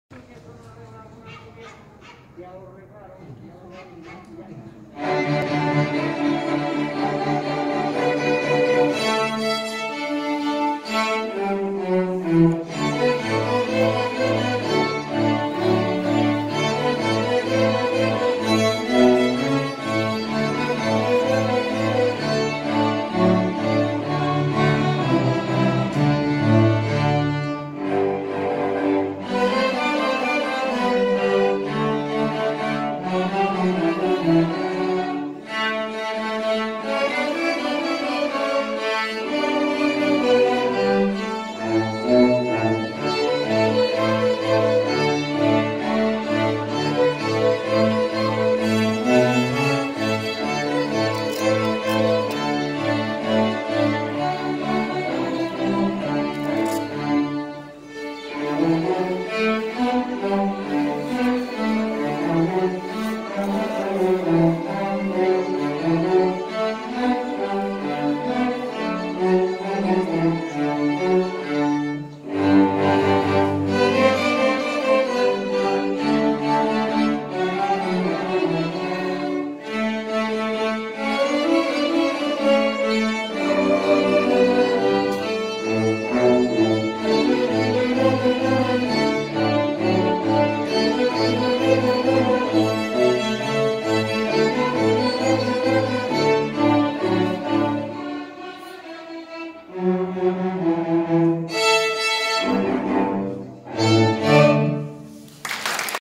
Orquestra EMM Martorell
El passat dissabte 2 d’abril l’orquestra de corda  de la nostra escola va  participar de la trobada d’orquestres que enguany es va celebrar a Olesa de Montserrat.